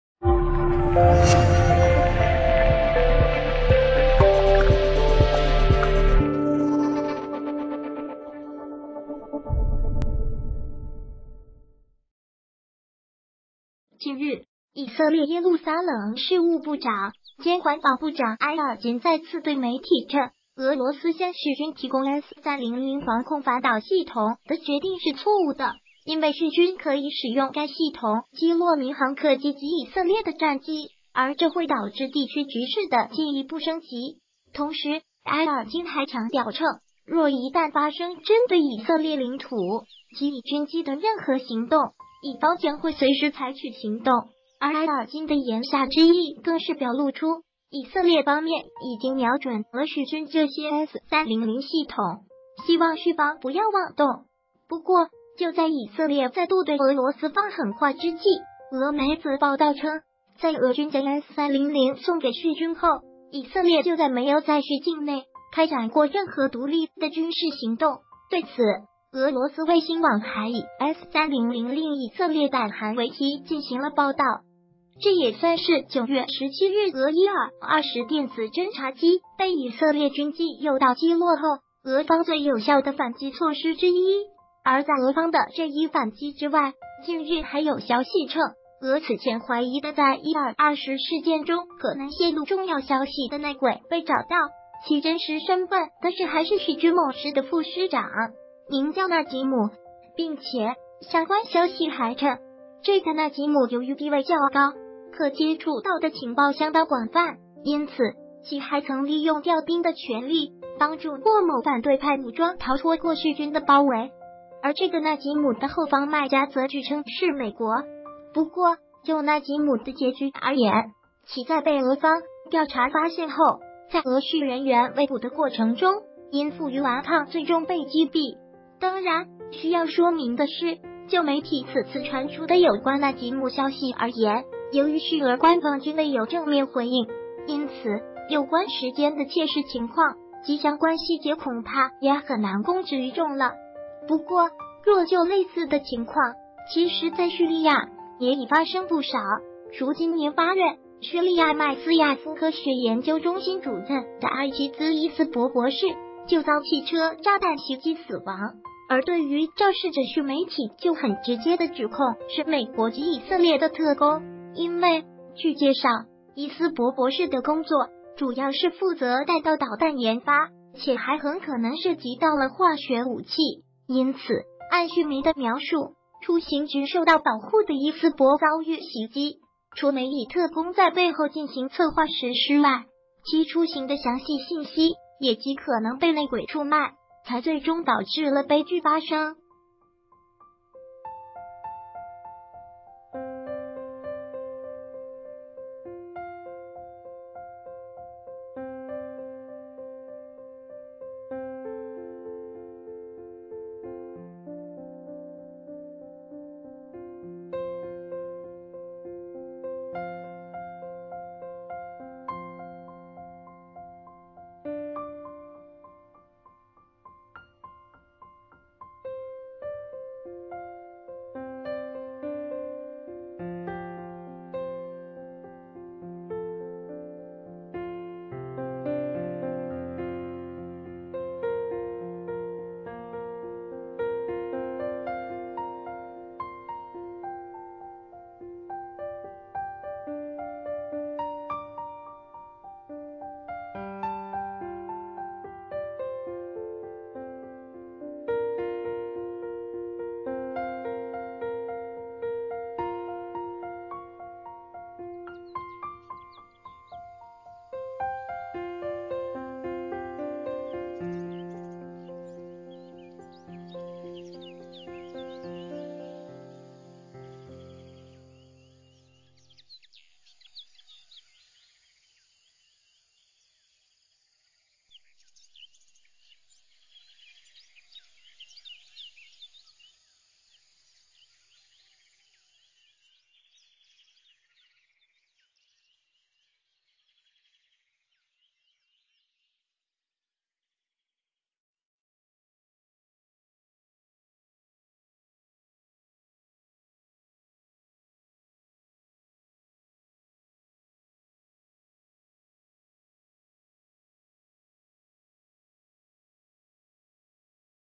叙军方高层卧底曝光，大批俄军惨死！反抗被俄军当场枪决
谢谢！近日，以色列耶路撒冷事务部长兼环保部长埃尔金再次对媒体称，俄罗斯向叙军提供S-300防空反导系统的决定是错误的，因为叙军可以使用该系统击落民航客机及以色列的战机，而这会导致地区局势的进一步升级。同时，埃尔金还强调称，若一旦发生针对以色列领土及以军机的任何行动，以方将会随时采取行动。而埃尔金的言下之意更是表露出，以色列方面已经瞄准了叙军这些S-300系统，希望叙方不要妄动。不过，就在以色列再度对俄罗斯放狠话之际，俄媒则报道称，在俄军将S-300送给叙军后，以色列就再没有在叙境内开展过任何独立的军事行动。对此，俄罗斯卫星网还以“S-300令以色列胆寒”为题进行了报道，这也算是9月17日俄伊尔-20电子侦察机被以色列军机“诱导”击落后，俄方最有效的反击措施之一。而在俄方的这一反击之外，近日还有消息称，俄此前怀疑的在伊尔-20事件中可能泄漏重要消息的“内鬼”被找到，其真实身份的是还是叙军某师的副师长，名叫纳吉姆。并且，相关消息还称，这个纳吉姆由于地位较高，可接触到的情报相当广泛，因此，其还曾利用调兵的权利，帮助过某反对派武装逃脱过叙军的包围。而这个纳吉姆的后方“卖家”则据称是美国。不过，就纳吉姆的结局而言，其在被俄方调查发现后，在俄叙人员围捕的过程中，因负隅顽抗最终被击毙。当然，需要说明的是，就媒体此次传出的有关纳吉姆消息而言，由于叙俄官方均未有正面回应，因此，有关时间的切实情况及相关细节恐怕也很难公之于众了。不过，若就类似的情况，其实在叙利亚也已发生不少，如今年8月，叙利亚迈斯亚夫科学研究中心主任的阿齐兹⋅伊思博博士就遭汽车炸弹袭击死亡，而对于“肇事者”叙媒体就很直接的指控是美国及以色列的特工。因为，据介绍，伊思博博士的工作主要是负责弹道导弹研发，且还很可能涉及到了化学武器。因此，按叙媒的描述，出行均受到保护的伊思博遭遇袭击，除美以特工在背后进行策划实施外，其出行的详细信息也极可能被“内鬼”出卖，才最终导致了悲剧发生。